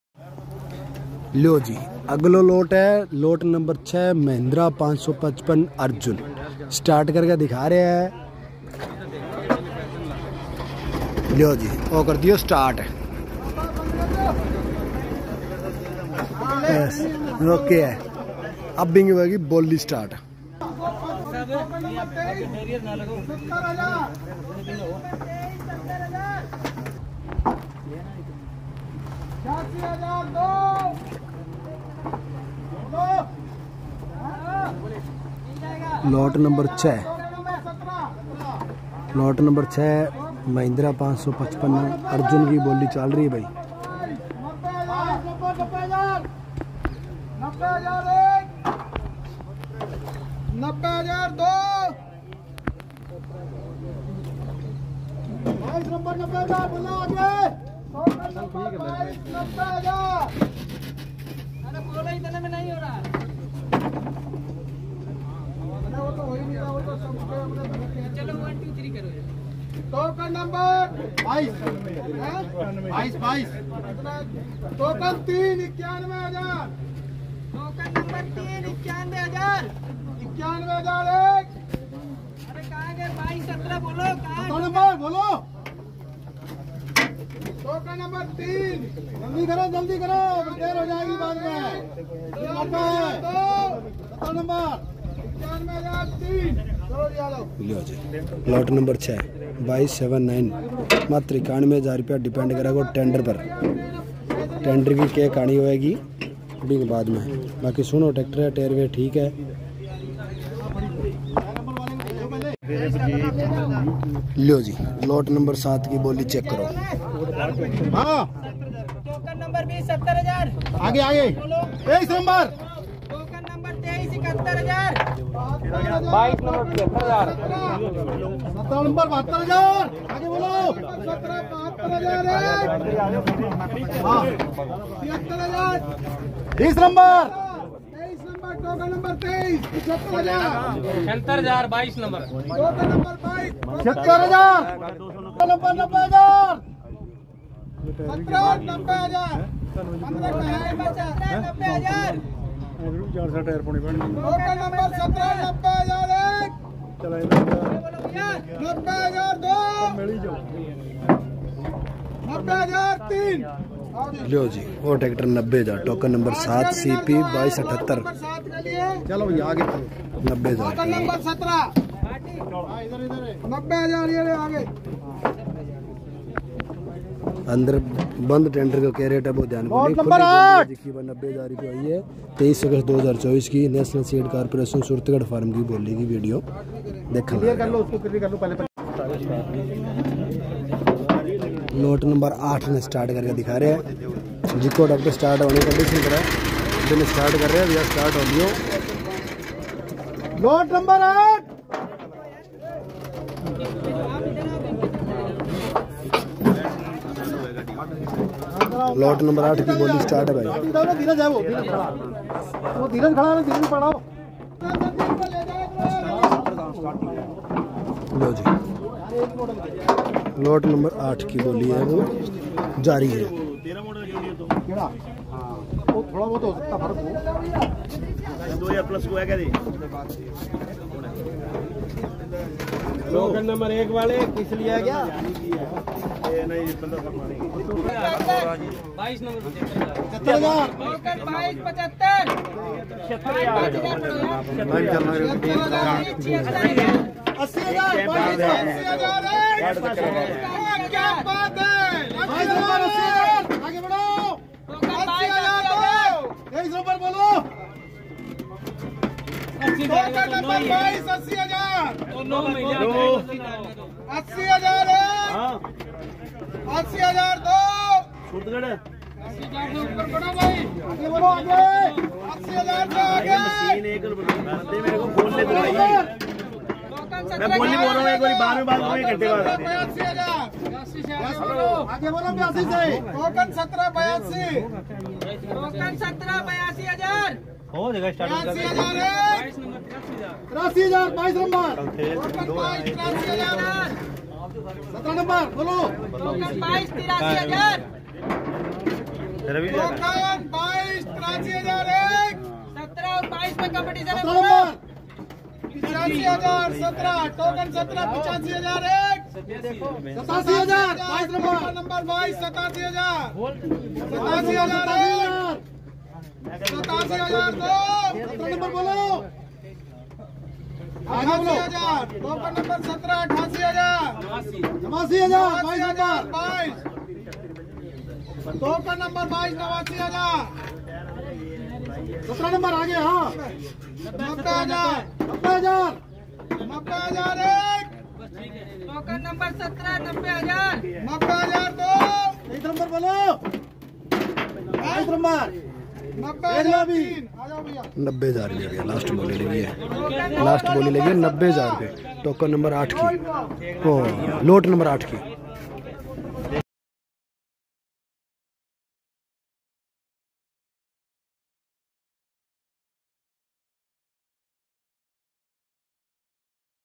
सूरतगढ़ फार्म में ट्रैक्टरों की नीलामी लॉट नंबर 6,7 और 8 Mahindra tractor Arjun 555
सूरतगढ़ फार्म में ट्रैक्टरों की नीलामी
 लॉट नंबर 6,7 और 8
Mahindra tractor Arjun 555
नेशनल सीड कॉरपोरेशन सूरतगढ़ फार्म बोली की वीडियो
National seed corporation Suratgarh form live auction tractor auction